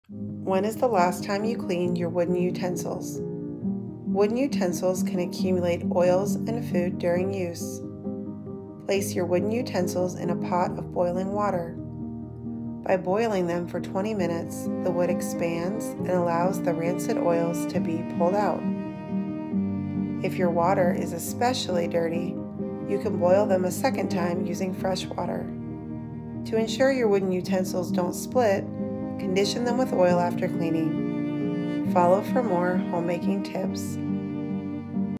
How to Clean Wooden Spoons
Check Caption for written Instructions👇👇👇👇

Whenever I am cooking something the first utensil I reach for is my favorite wooden spoon.

I can easily go a year or more without deep cleaning my wooden spoons.

It is important to make sure you are cleaning your wooden utensils so that rancid oils and food do not accumulate.

-Collect your wooden utensils
-Place them into a pot of boiling water
-Boil for 20 minutes
-Take them out and let them air dry
-Condition them with oil to prevent them from cracking or splitting